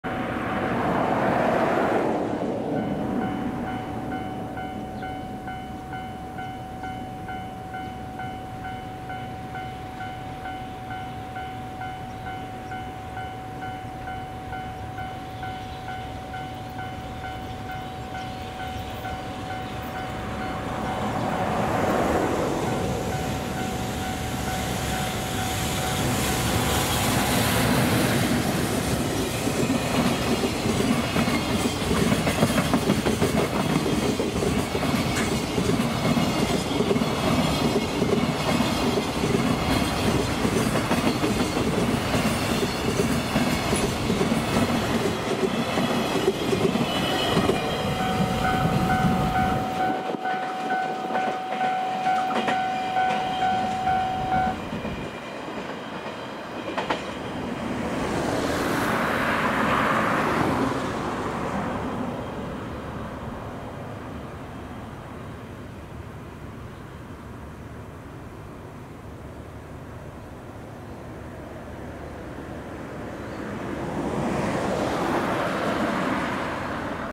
鶴桃【4K】山陽本線 上り 貨物列車 1062レ EF210-135号機牽引 コンテナ15B JR貨物 新鶴見機関区 糸崎 2020.7.12 M2400014
1062列車 幡生～名古屋 新鶴見機関区 EF210 A214仕業